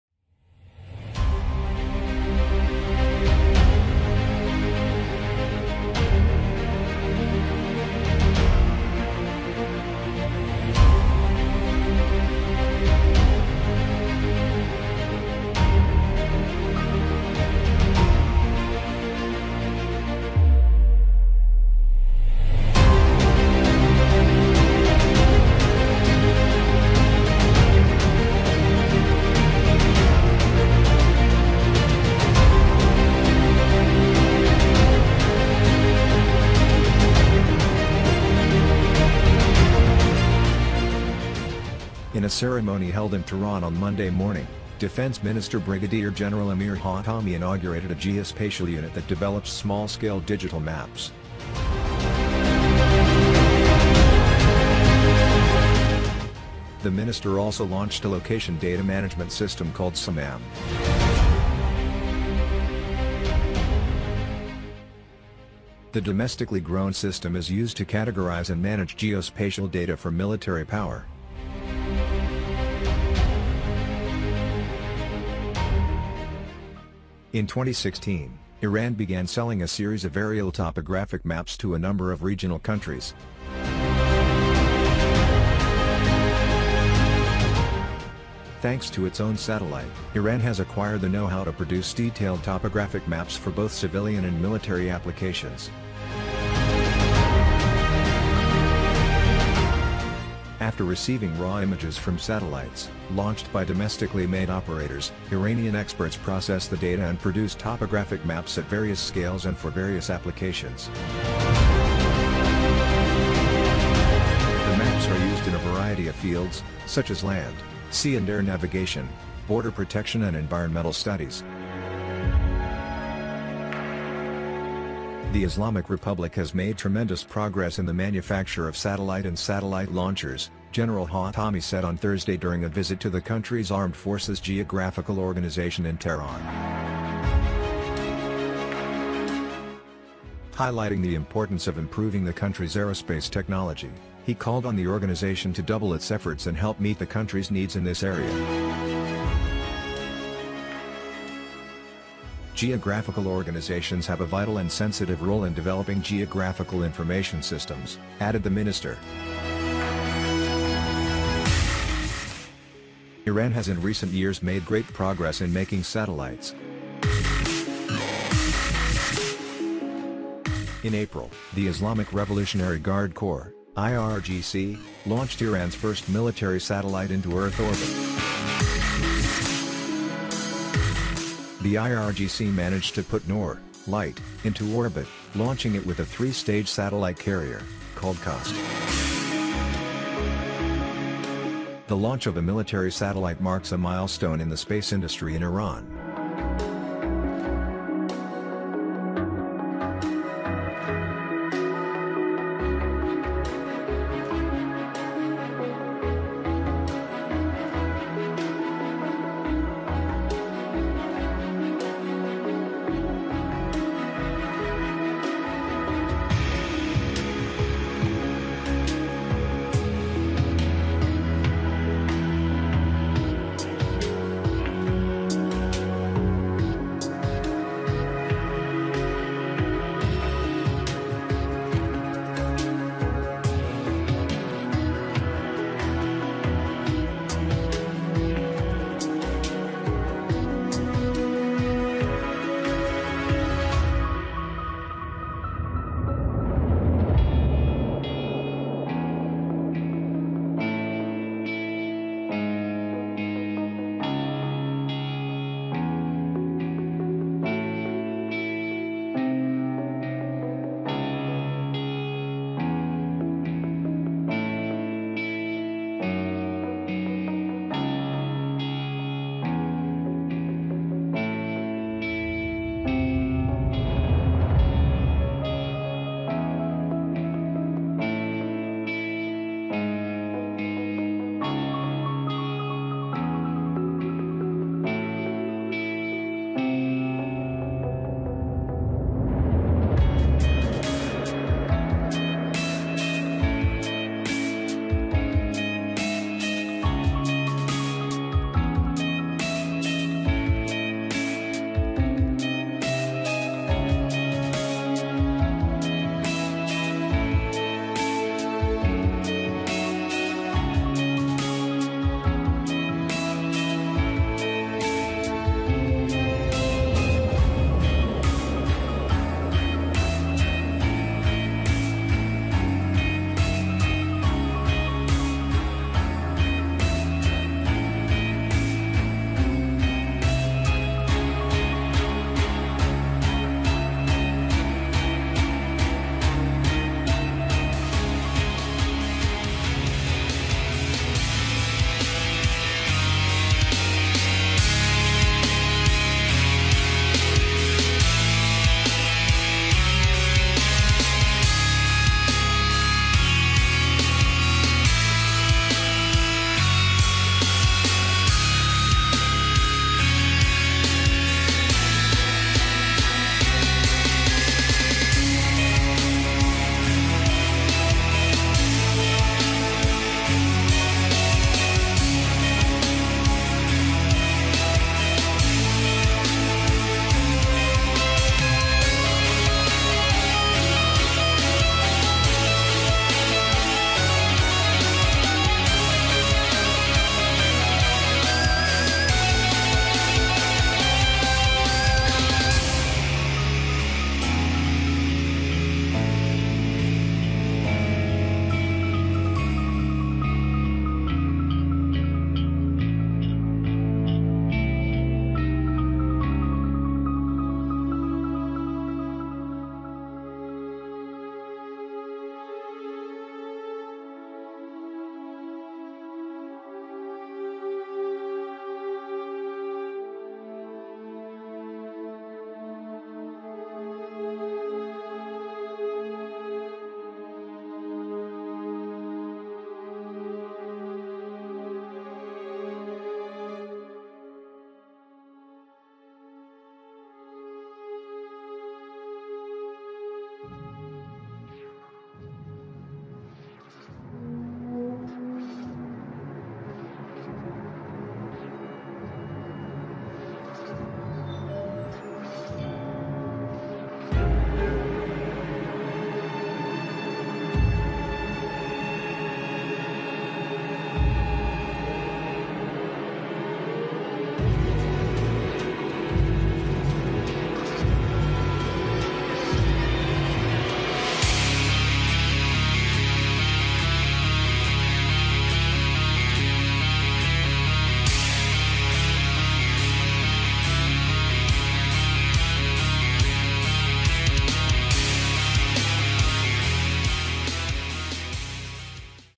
Iran launched geographic products and Military satellite technology
In a ceremony held in Tehran on Monday morning, Defense Minister Brigadier General Amir Hatami inaugurated a geospatial unit that develops small-scale digital maps.
The Minister also launched a location data management system called 'Samam'.
The domestically grown system is used to categorize and manage geospatial data for military power.
Thanks to its own satellite, Iran has acquired the know-how to produce detailed topographic maps for both civilian and military applications.
After receiving raw images from satellites, launched by domestically made operators, Iranian experts processed the data and produced topographic maps at various scales and for various applications.